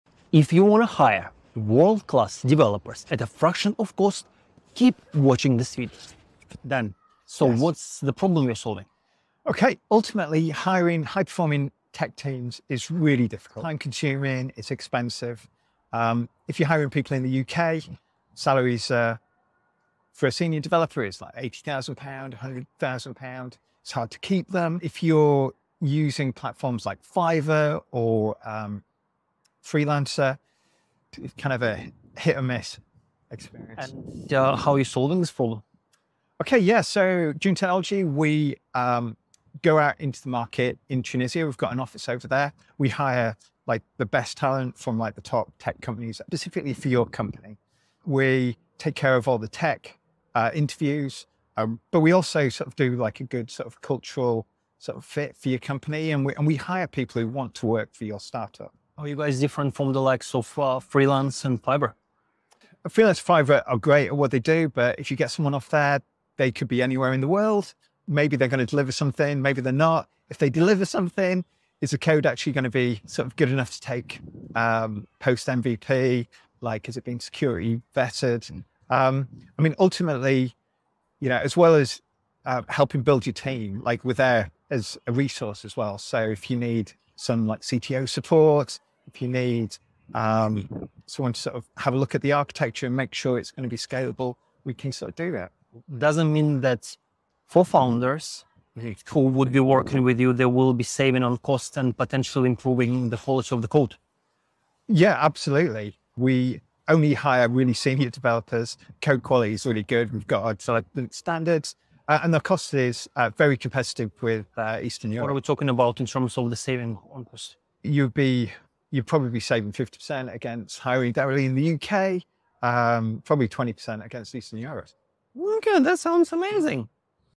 Breaking the Hiring Cycle: Offshore Outsourcing Solutions with Daniel Atherton | Dune Technology Ltd
🚀 Introducing Daniel Atherton from Dune Technology Ltd, offering a smart solution to one of the biggest challenges faced by startups: hiring a high-performing tech team.

👨‍💻 The Challenge:
- Startups often struggle to compete with large corporates for quality tech resources, especially before securing investments.

💡 The Solution:
- Offshore outsourcing provides an affordable and effective way to build a great development team, creating job opportunities in regions where job security is a challenge.

🌍 Benefits:
- Access to a wider talent pool
- Cost-effective development solutions
- Ability to scale quickly and efficiently

📹 Watch our video for more details on how offshore outsourcing can benefit your startup.